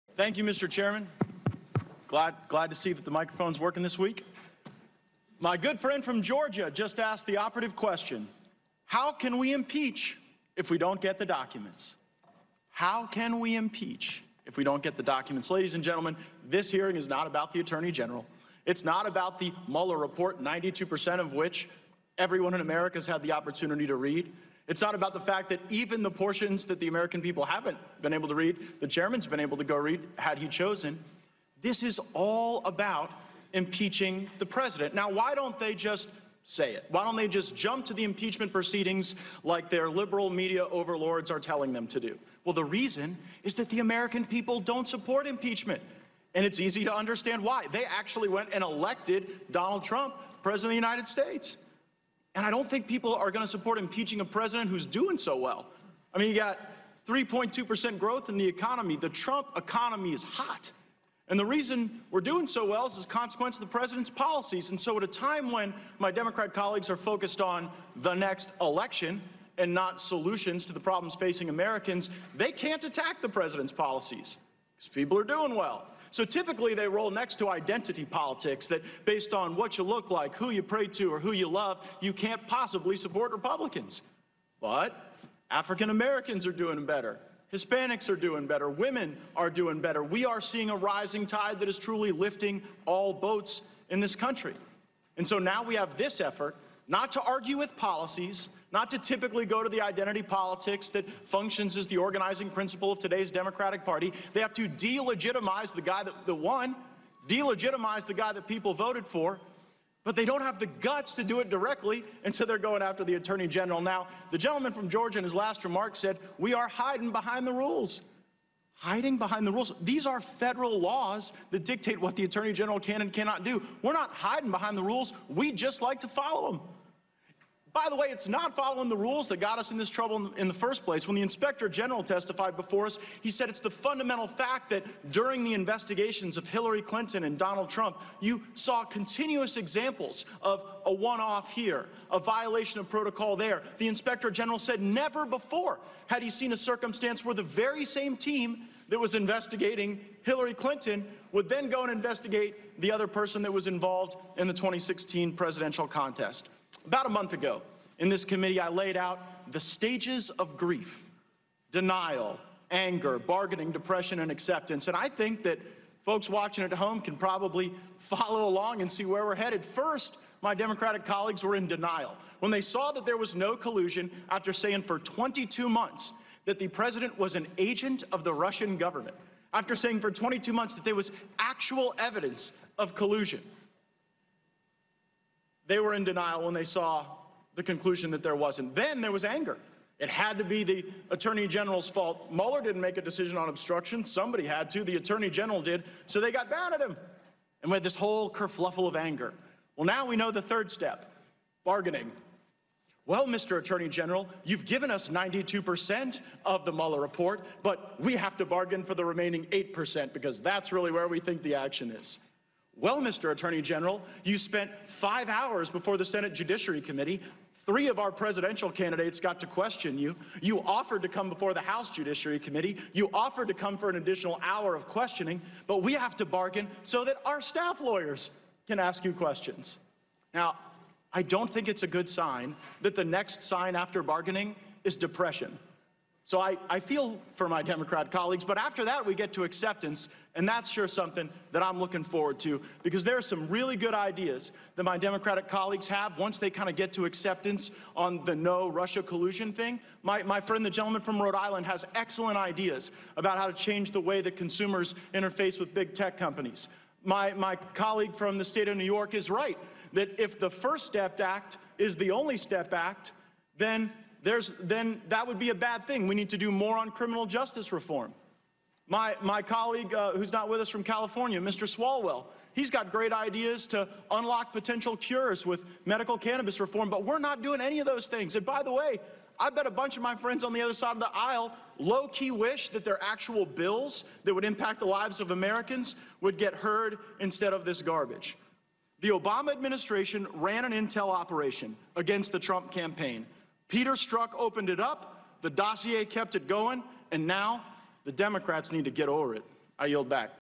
Gaetz Speaks Against Holding Barr in Contempt
Congressman Gaetz speaks against holding Attorney General William Barr in contempt of Congress.